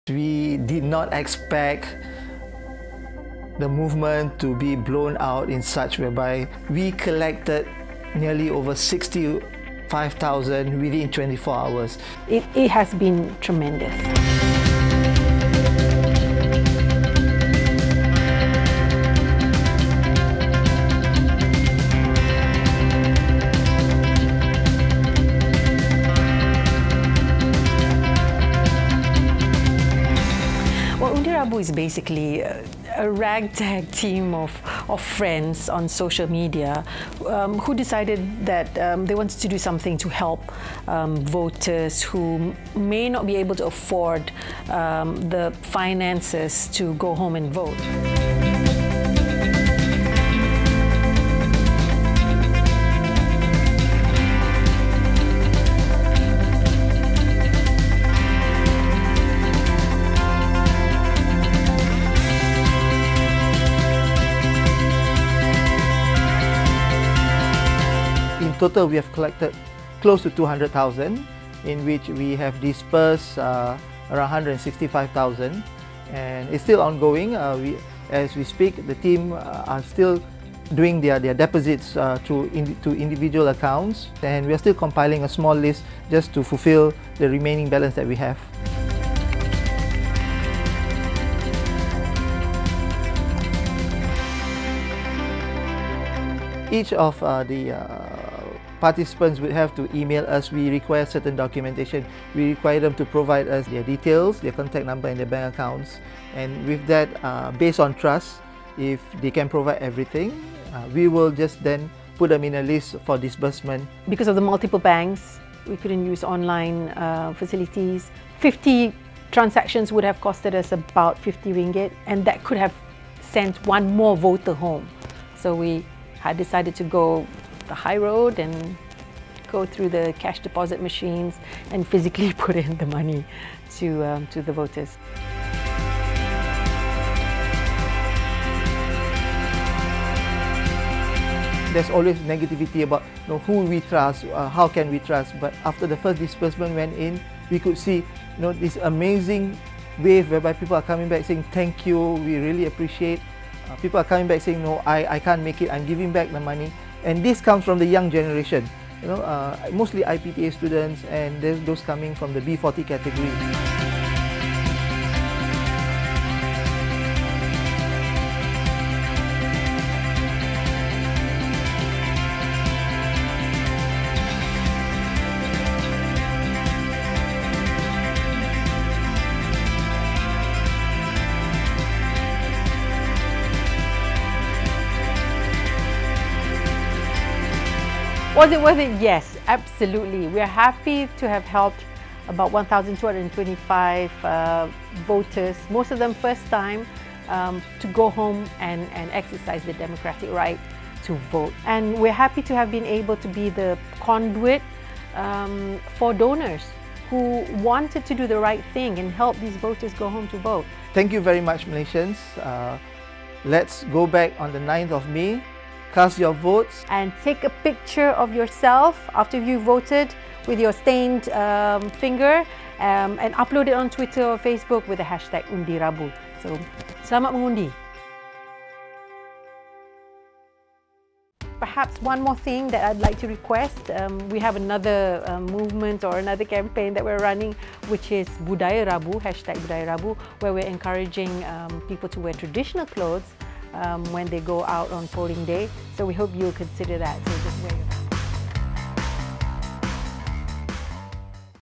UndiRabu: RM200K raised to help voters get home for GE14
A group of friends managed to raise nearly RM200,000 to help voters travel home for GE14.

The social media initiative called UndiRabu has benefited more than 1,200 voters.